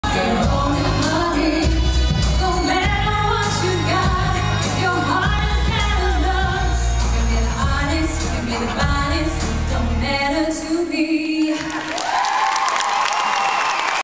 College Graduation
This Video has been transferred from Google Videos, The original date of this video is; 12th July 2006

This is normally EXTRMLLEY boring I never like going but this year I got a surprise as you can see hehe.